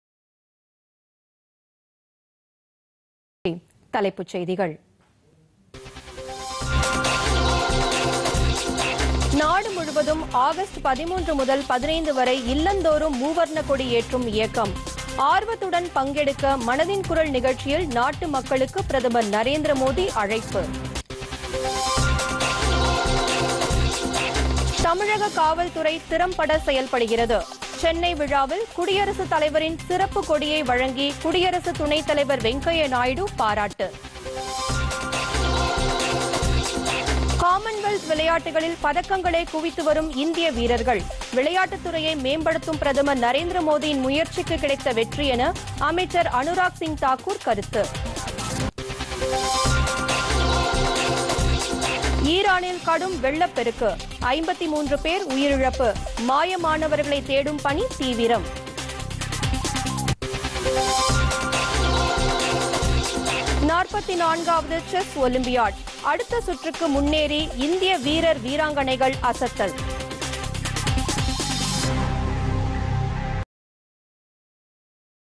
தலைப்புச்செய்திகள் - பகல் 2 மணி (31 - 07 - 2022)
1)     நாடு முழுவதும் ஆகஸ்ட் 13 முதல் 15-வரை இல்லந்தோறும் மூவர்ணக்கொடி  ஏற்றும் இயக்கம் - ஆர்வத்துடன்  பங்கெடுக்க மனதின்குரல் நிகழ்ச்சியில் நாட்டு மக்களுக்கு பிரதமர் நரேந்திரமோதி அழைப்பு

2)    தமிழக காவல்துறை  திறம்பட செயல்படுகிறது - சென்னை விழாவில்  குடியரசுத்தலைவரின் சிறப்பு கொடியை வழங்கி குடியரசுத்துணைத்தலைவர் வெங்கய்யா நாயுடு பாராட்டு

3)  காமன்வெல்த் விளையாட்டுகளில் பதக்கங்களை குவித்து வரும்  இந்திய வீரர்கள் - விளையாட்டுத்துறையை மேம்படுத்தும் பிரதமர் நரேந்திரமோதியின் முயற்சிக்கு கிடைத்த வெற்றி என அமைச்சர் அனுராக்சிங் தாக்கூர் கருத்து

4)   ஈரானில் கடும் வெள்ளப்பெருக்கு -  53 பேர் உயிரிழப்பு  - மாயமானவர்களை தேடும் பணி தீவிரம்

5)   44-வது செஸ் ஒலிம்பியாட் : அடுத்த சுற்றுக்கு முன்னேறி இந்திய வீரர் - வீராங்கனைகள் அசத்தல்